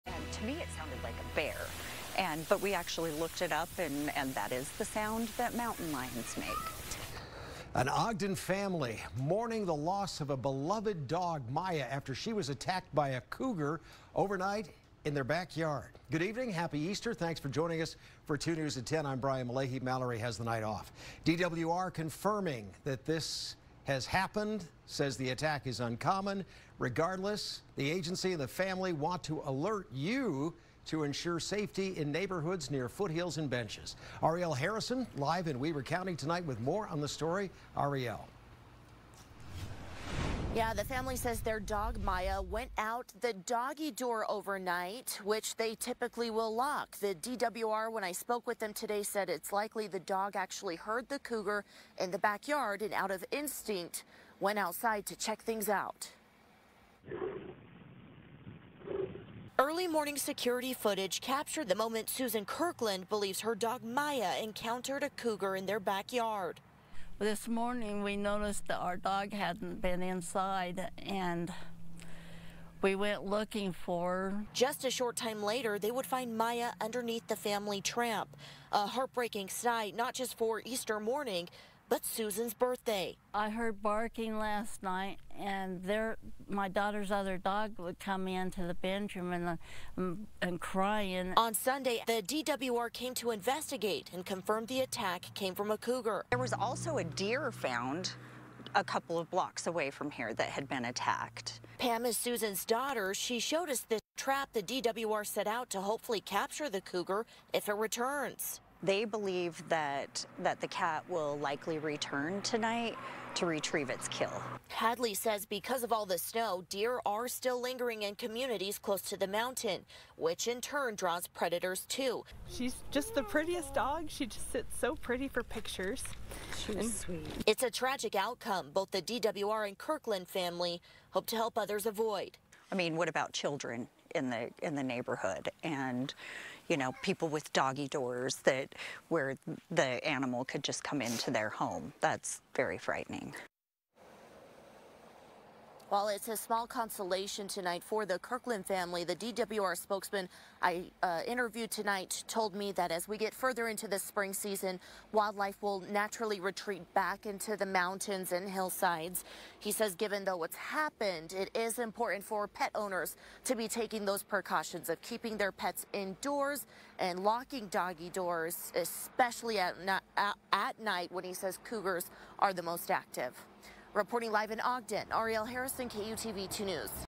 Family mourning loss of husky killed by cougar in Ogden backyard
OGDEN, Utah (KUTV) — An Ogden family is left reeling from the sudden loss of their cherished dog, a 10-year-old husky, after she was attacked and killed by a cougar in their backyard overnight.

The Kirkland family told 2News they believe Miya, being a natural protector, heard the cougar prowling and ventured outside through the doggy door, which is typically locked.

Sadly, the incident occurred in the early hours of Sunday morning, when the family was asleep and unaware of what was happening. They are now grappling with their heartbreak but also a deep sense of responsibility to alert others about the danger lurking near the foothills and benches